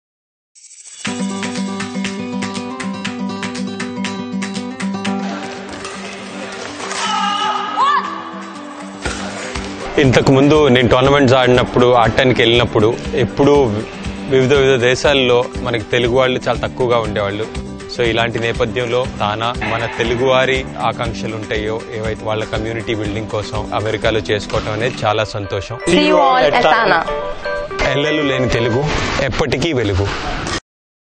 Gopi Chand - Badminton Coach Invites every one to TANA Conference 2017
The TANA has started the preparations to celebrate the TANA convention in 2017 and this event will be celebrated grandly on 26, 27 and 28 of May 2017 at Saint Louis. The TANA committee has met to conduct this convention with much grandeur under the supervision of Dr Jamapala Choudhary. The TANA will also be crossing a milestone of 40 years through this convention.

1. Desi Plaza Web Portal
4. MastiTime Radio - IOS app
5. MastiTime Radio - Android app